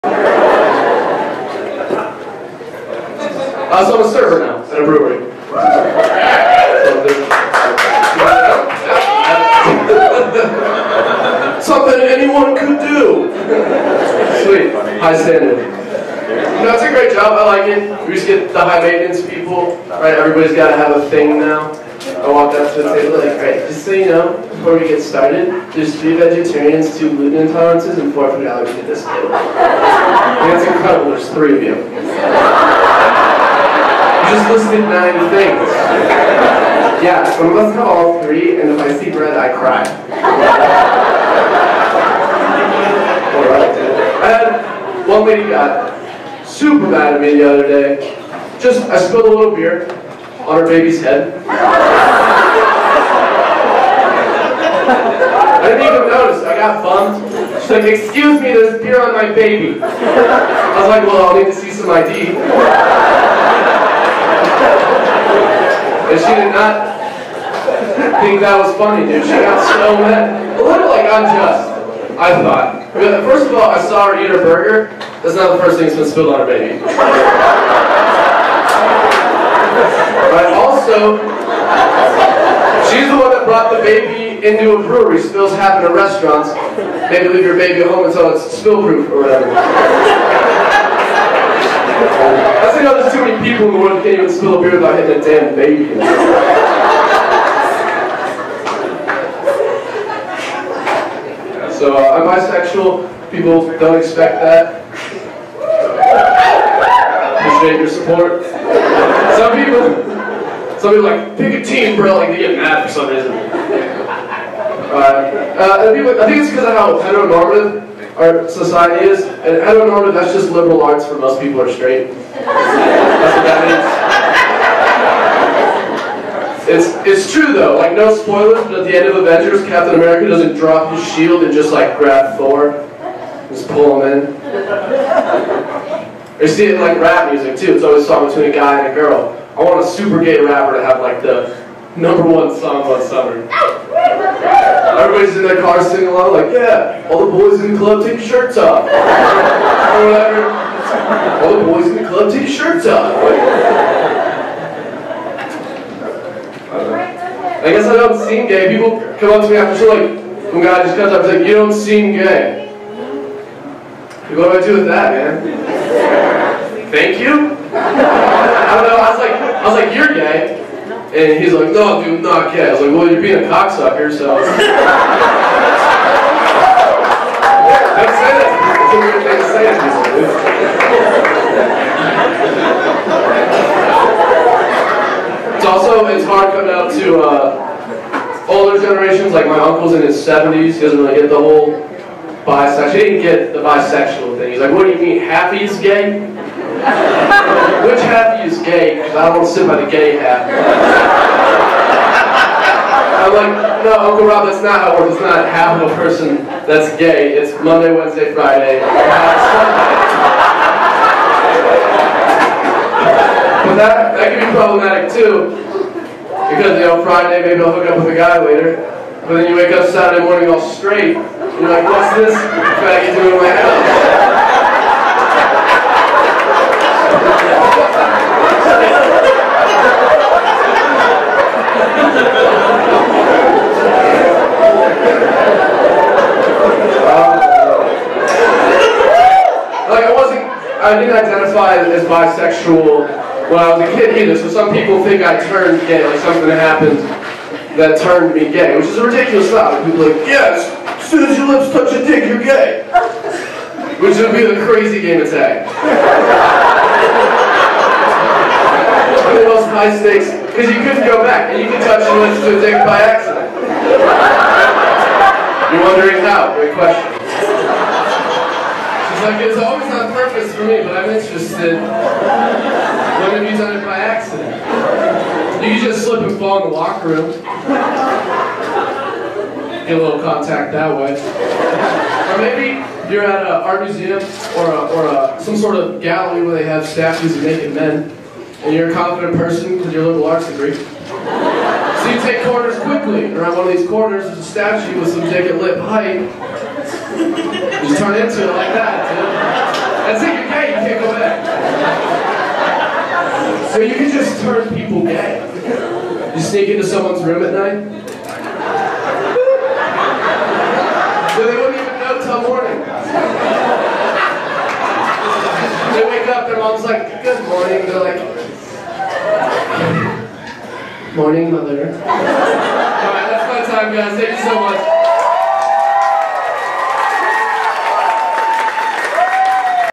Elliot Weber - 5 Minute Set
Stand-up set perfromed at Next Best Comic of the Four Corners comedy competition. 05.04.18